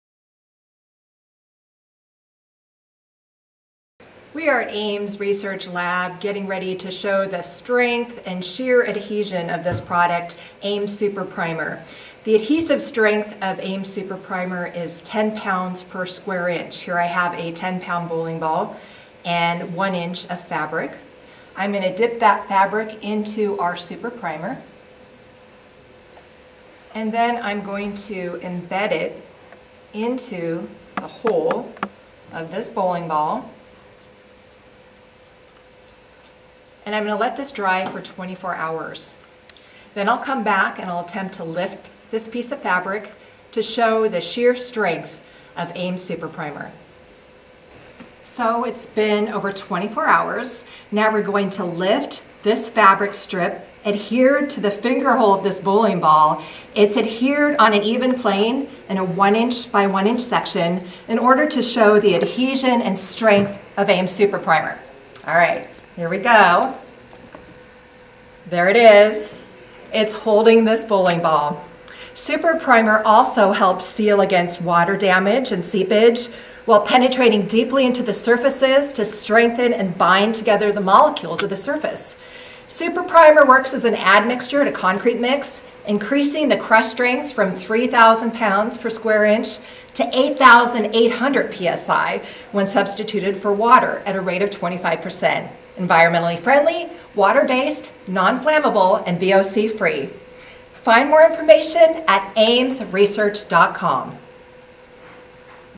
Ames® Super Primer™ - Strong Enough to Hold a Bowling Ball?
Is it strong enough to hold up against the strength of a 10 pound bowling ball?  We put it to the test!!
Super Primer™ can be used for concrete, brick, tile, EPDM rubber, stucco, wood, metal, and many hard-to-stick surfaces. When added to concrete at a rate of 25% substitution for water - ATSM test standards show - Super Primer™ will strengthen the crush strength from 3000 PSI to 8800 PSI.

Super Primer™  leaves a clear and slightly tacky surface that substantially increases adhesion for Ames® coatings. Much more powerful than a water repellent, Super Primer™ seals against water damage and seepage while penetrating deeply into the surface to strengthen and bind together the molecules of the surface it is applied to. Super Primer™ helps stop concrete efflorescence, and blocks most gasses penetrating concrete, mortar, or stucco. Super Primer™ is elastic and highly resistant to cracking and peeling as the surface expands and contracts, even at -35 degrees F. It is easy to use, environmentally friendly, water-base, nearly odorless, non-flammable, and VOC free. Simply brush, roll or spray. Clean up with water.